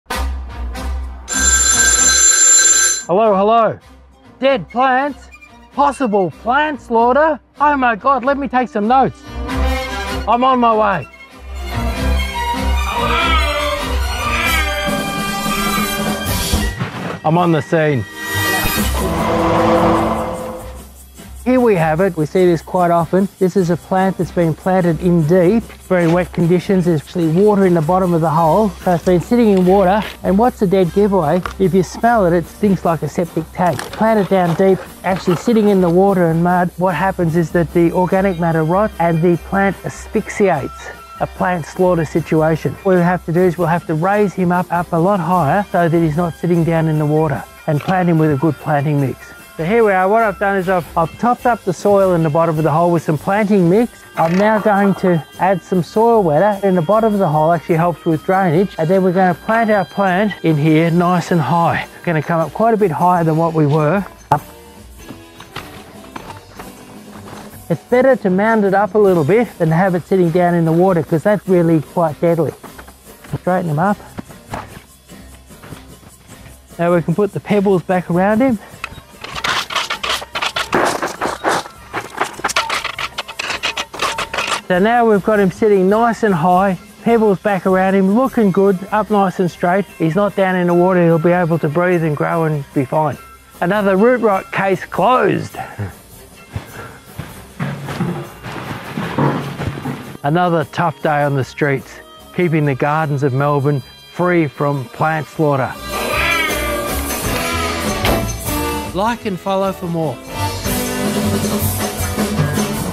Detective Jack Aranda on the Crime Scene of a Water Logged Plant!
Detective Jack Aranda (aka Chris Lucas) shows you how to solved a bogged plants situation, where a plant has been fed too much water and cannot drain it from the clay soil from around it. A simple how to on how to make a plant healthy and happy in the poor soil quality of Melbourne, Victoria.

Visit us at 1477 Sydney Road, Campbellfield, 3061, Victoria, Australia